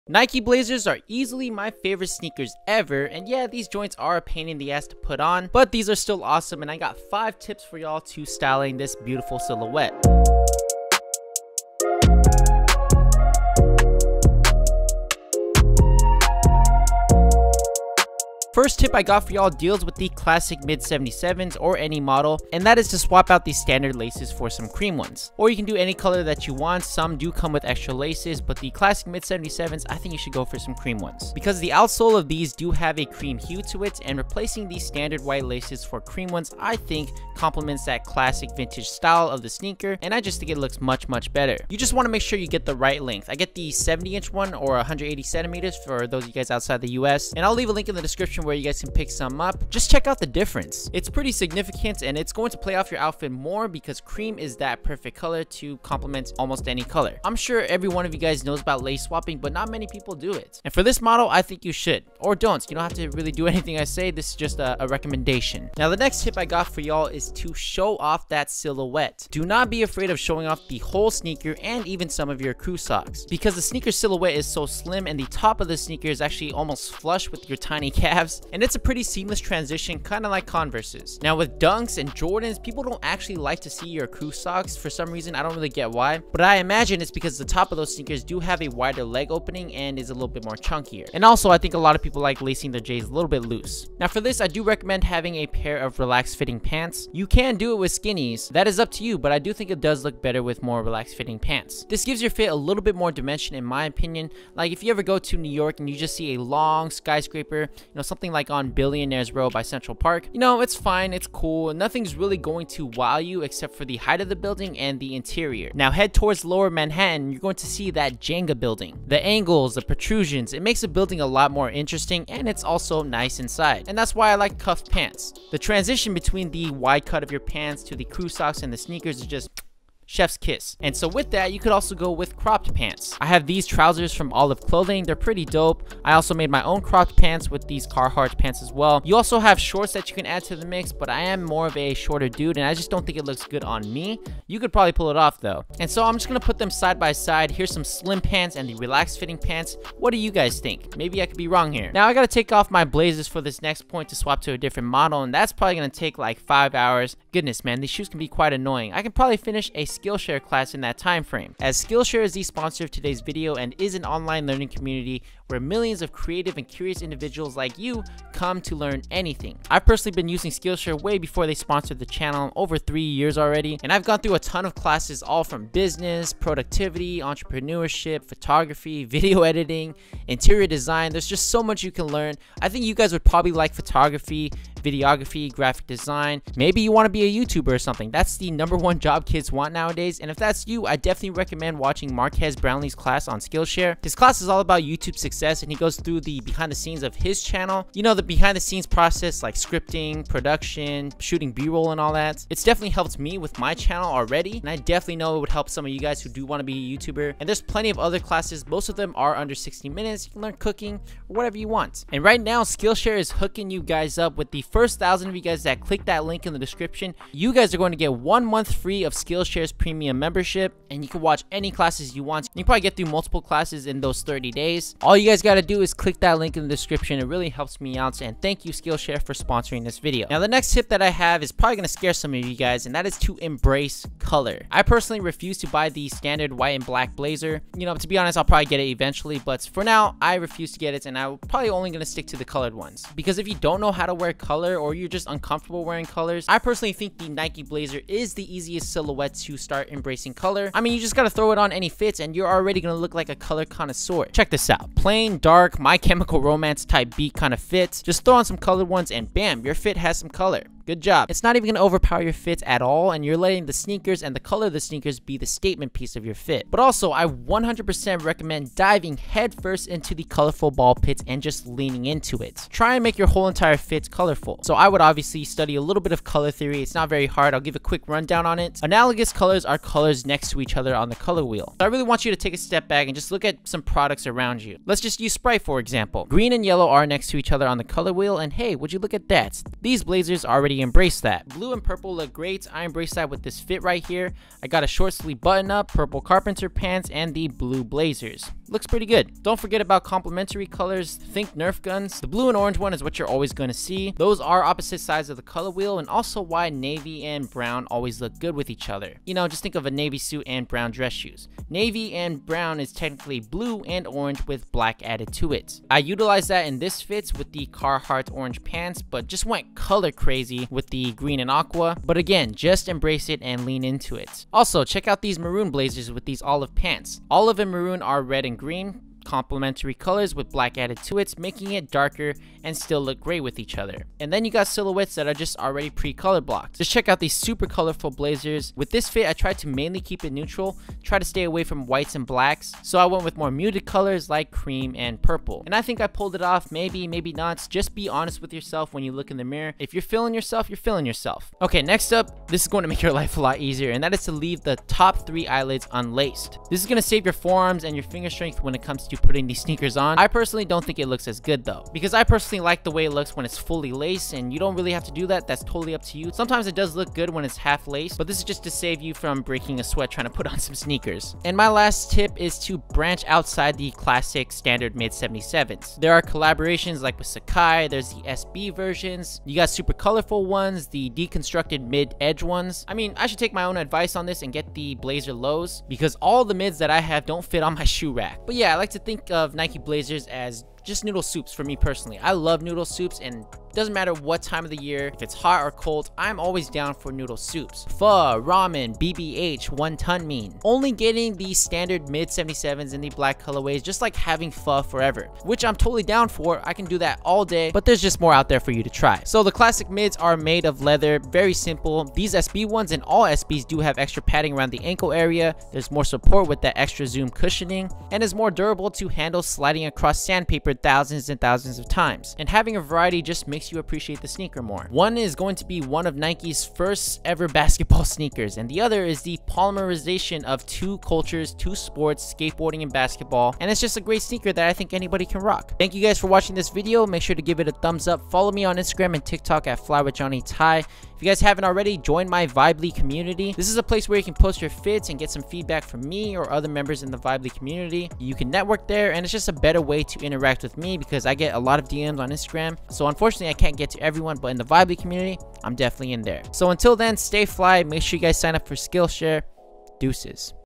How to Style ANY Nike Blazers (SB/Mid 77')
Nike Blazer Mid 77'
Nike SB Blazer (Better quality/for skaters)
Cream Laces

In this video I will give some tips to style the nike blazer mid 77' as well as pretty much any other model like the sb blazer and sb blazer mid edge for example.